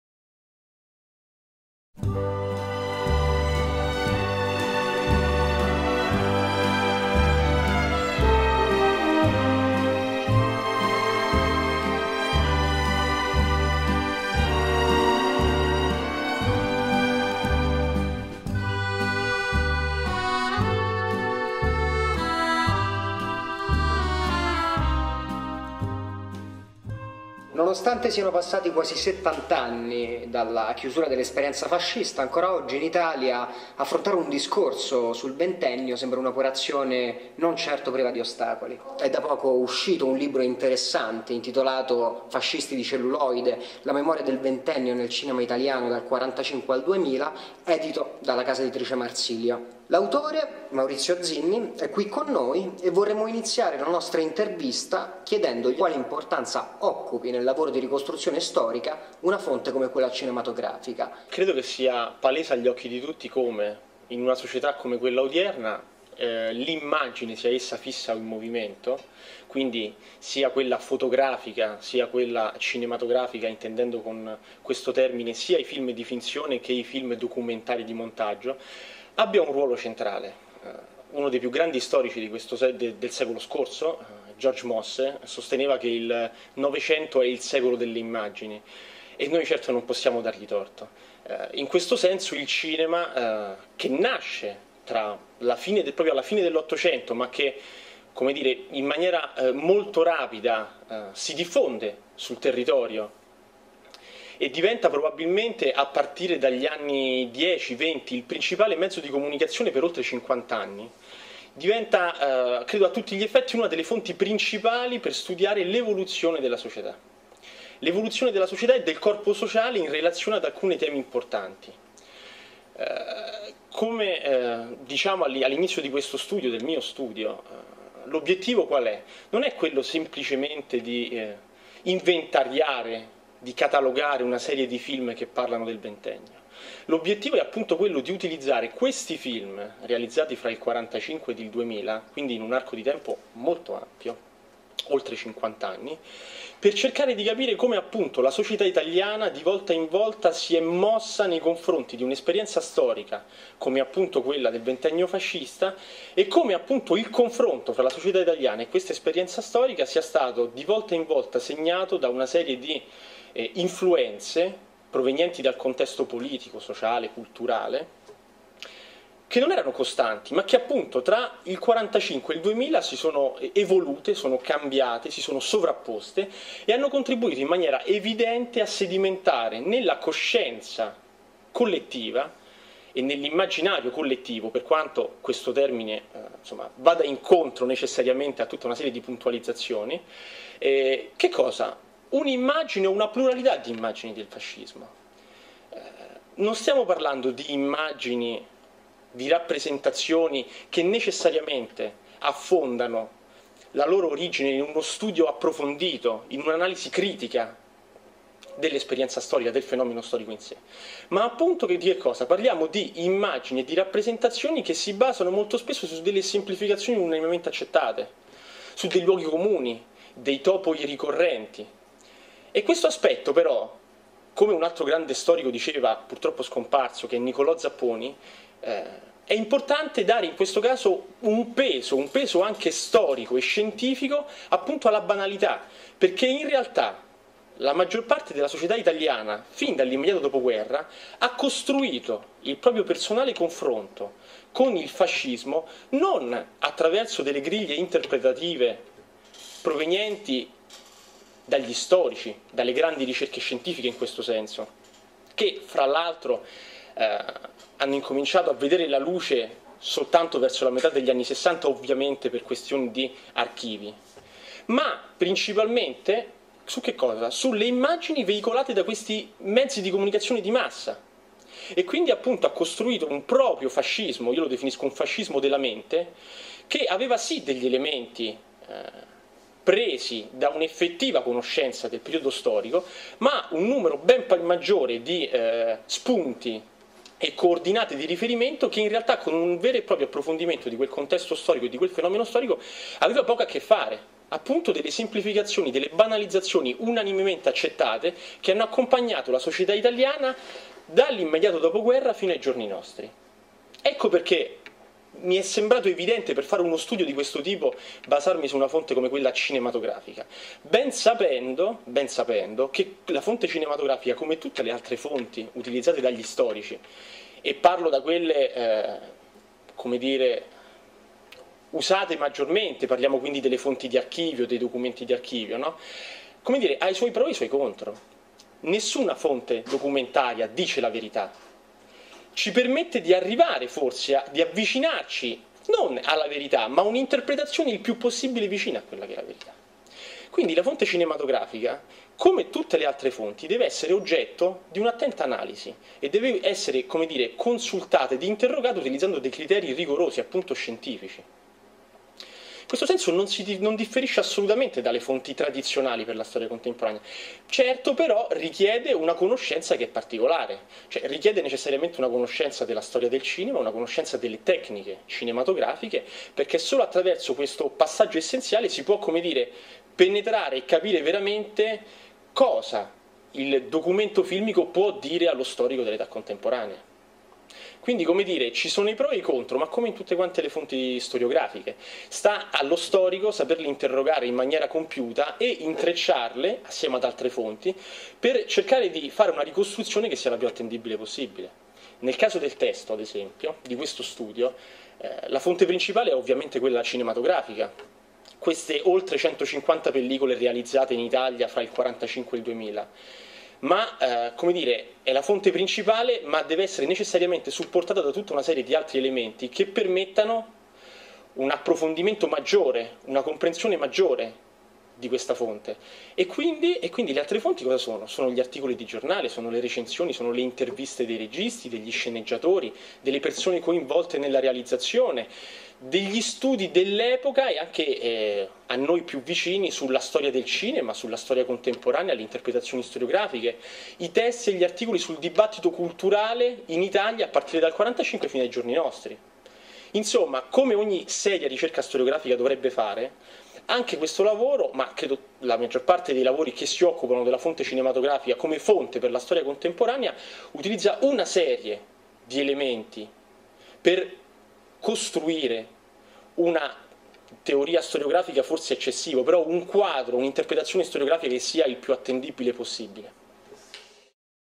MAURIZIO ZINNI, FASCISTI DI CELLULOIDE (1/7)
Maestri&Compagni intervista Maurizio Zinni sul suo ultimo libro "Fascisti di Celluloide. La memoria del ventennio nel cinema italiano (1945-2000)". Intervista a cura di Paolo Granata e Roberto Pierri. Video di Davide Maria Marucci.

Seguiteci sulla nostra pagina Facebook per essere sempre aggiornati sui nuovi video